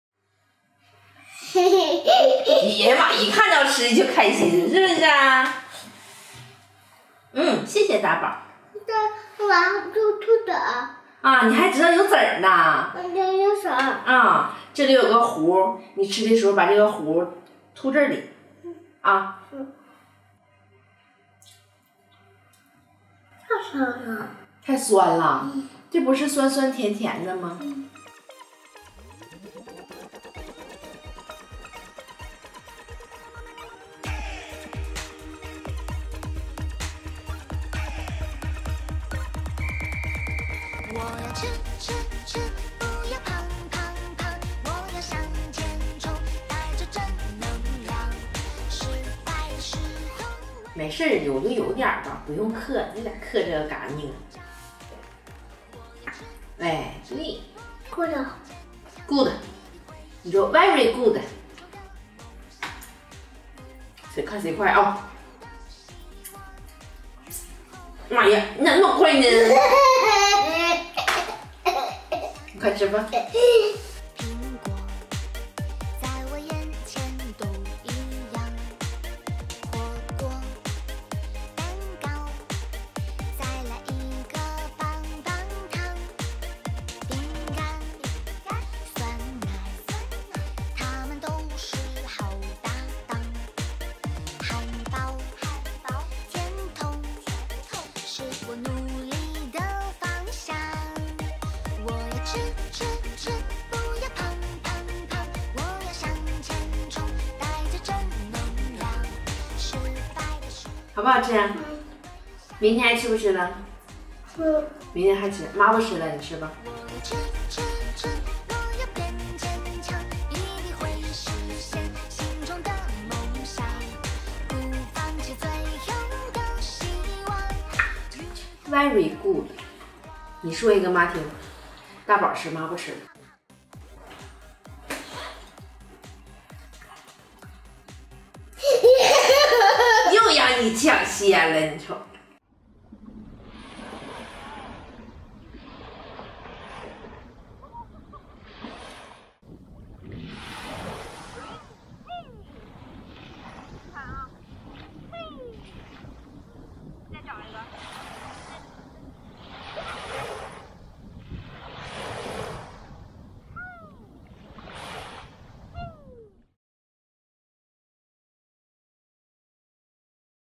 突发疫情，幼儿园停课，宸宸在家吃间食，妈妈准备了新鲜杨梅【小鸡嘎嘎香】
这里是小鸡嘎嘎香的官方YouTube频道🐔！
欢迎来了解我的家庭美食食谱，教你如何在家做出让一家老小都吃爽的美味😋~